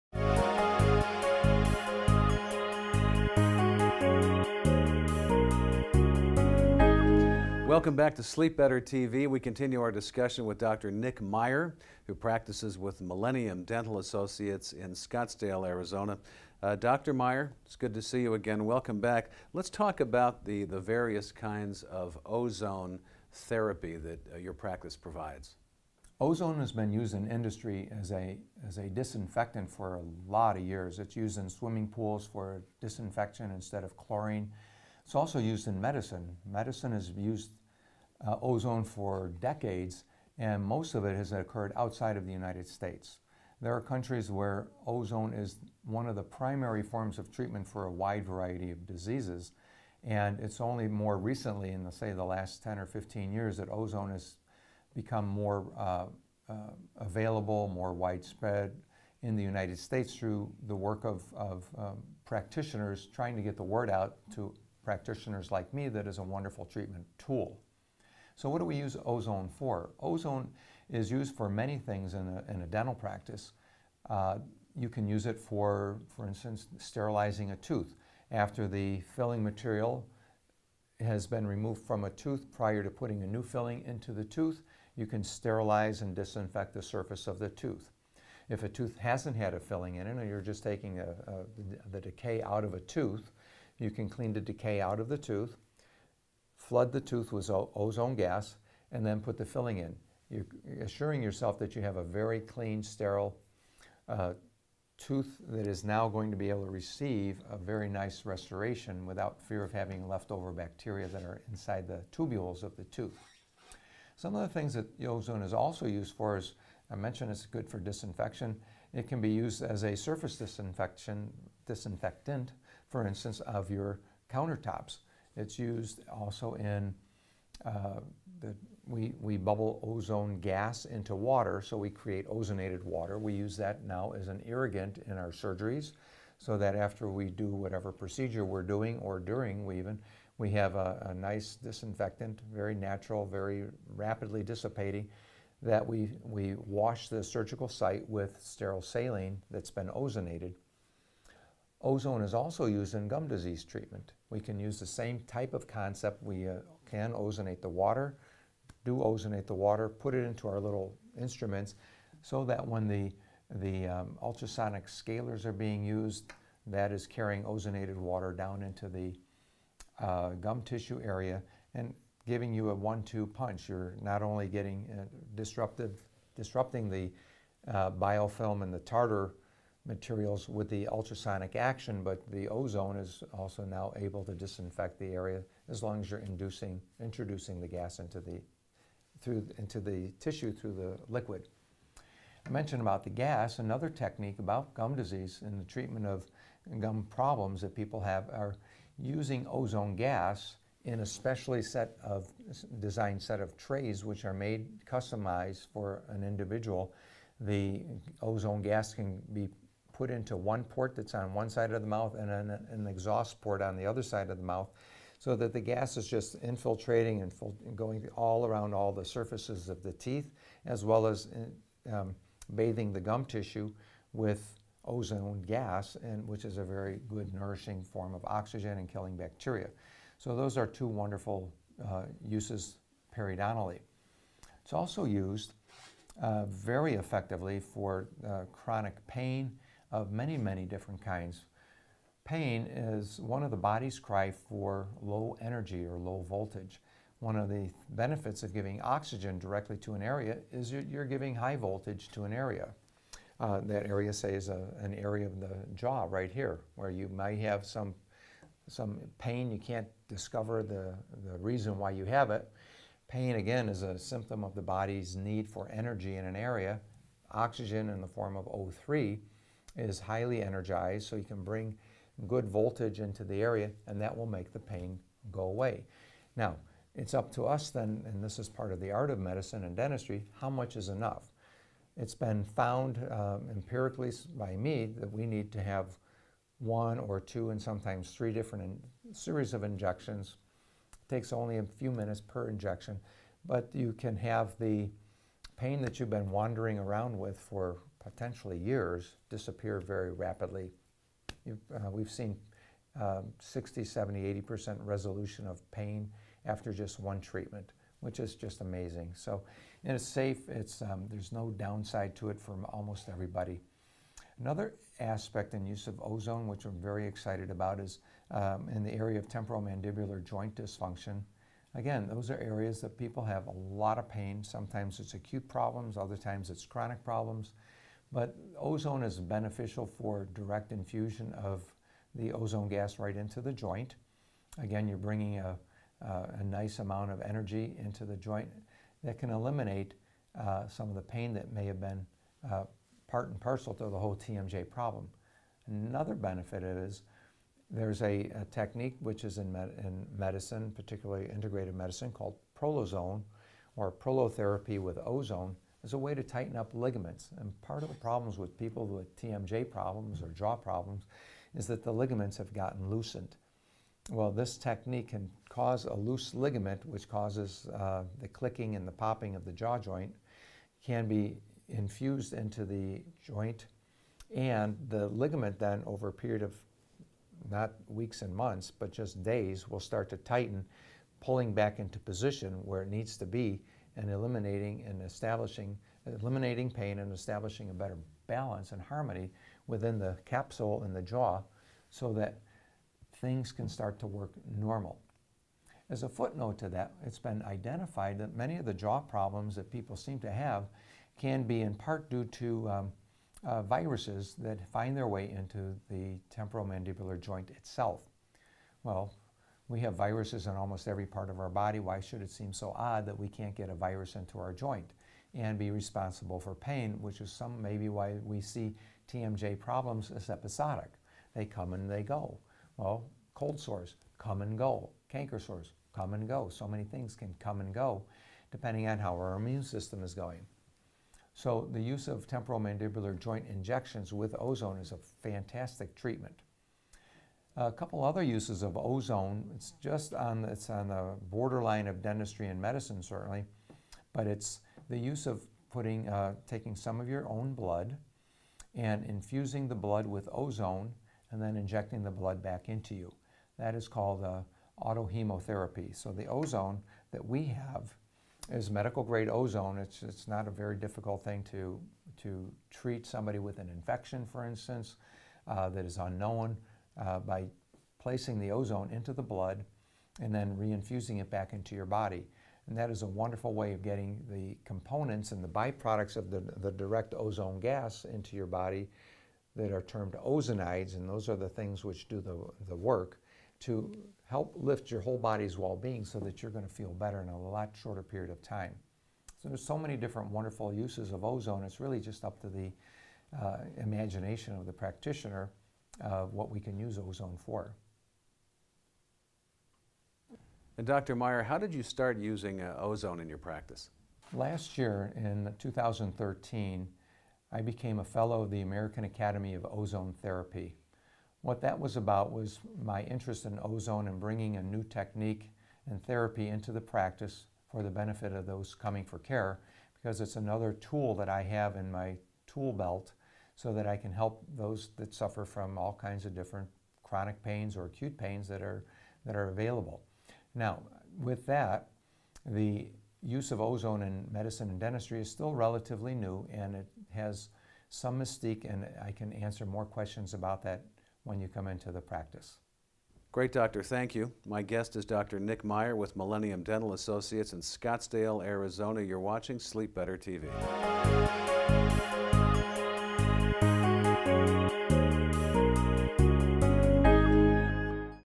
What is Ozone Therapy?
Dr. Nicholas Meyer of Millennium Dental Associates in Scottsdale, AZ explains how ozone therapy is applied in his dental practice.